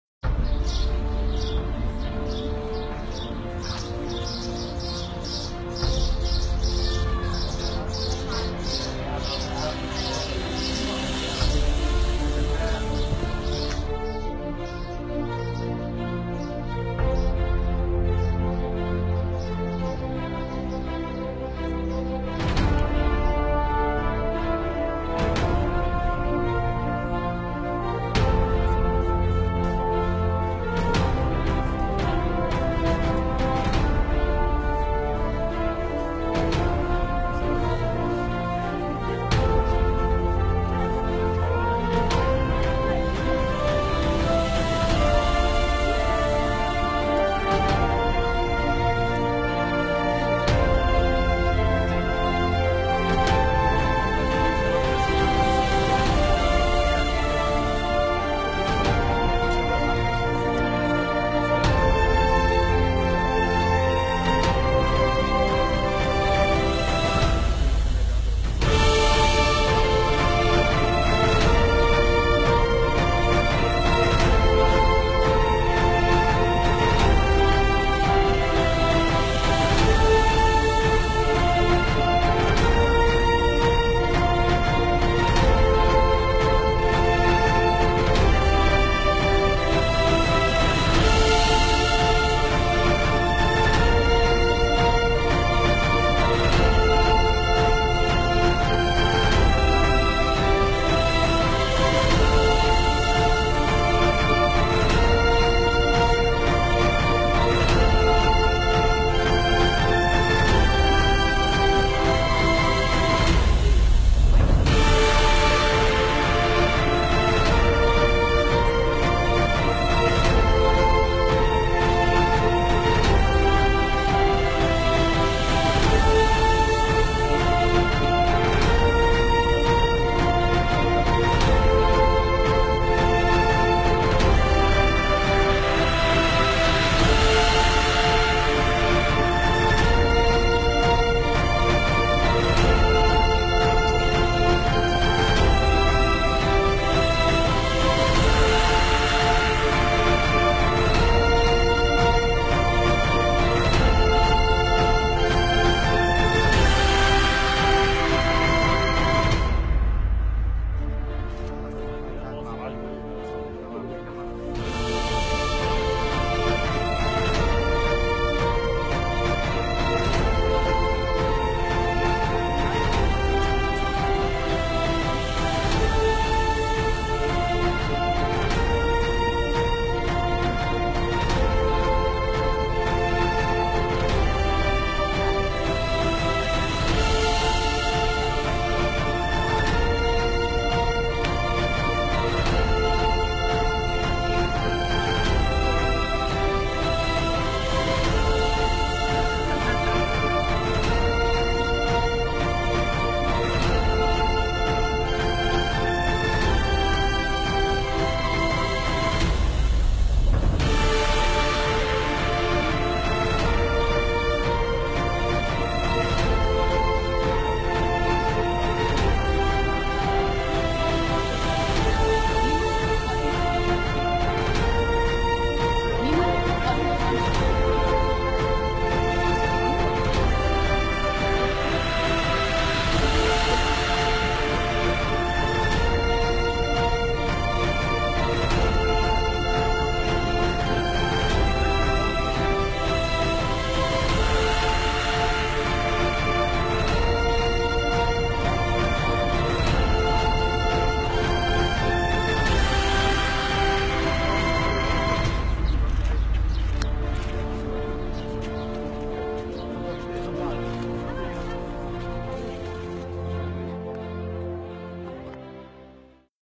HALKININ İÇİNDE KORUMASIZ GEZEN BEŞŞAR ESAD!!!
Adam Ülkesinde Halkının içinde gezerken yanında koruma Ordusu yok!!!